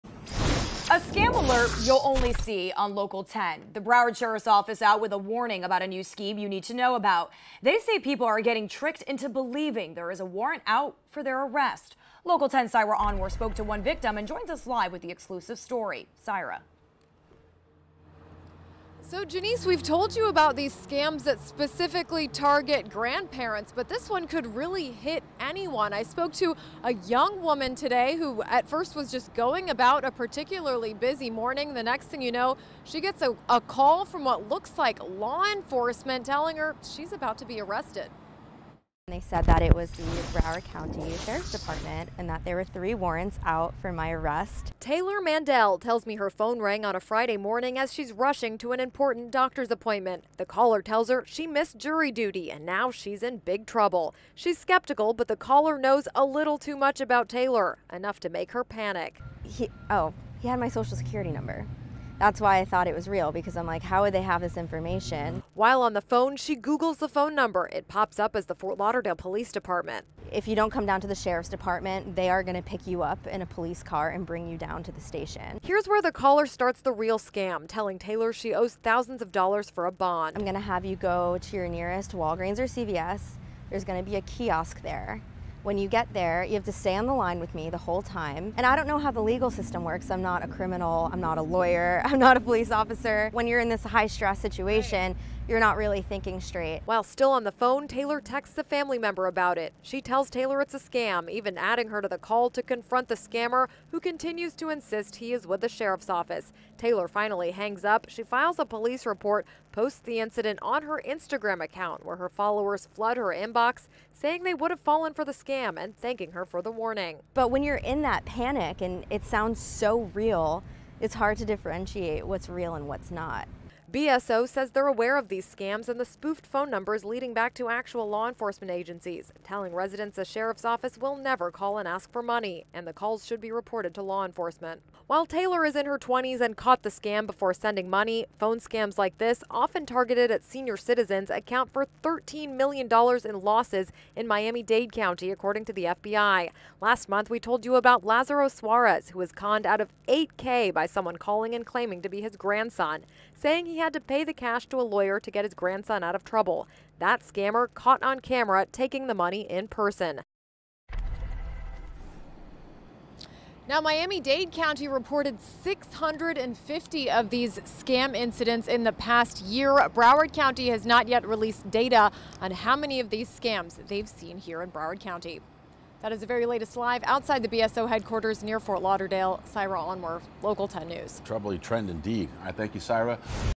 BSO warns of scam callers threatening arrests if payments aren't made
The Broward Sheriff’s Office is warning the public about scammers who are calling people of all ages, claiming to be from local police departments and asking for money to put an arrest warrant on hold.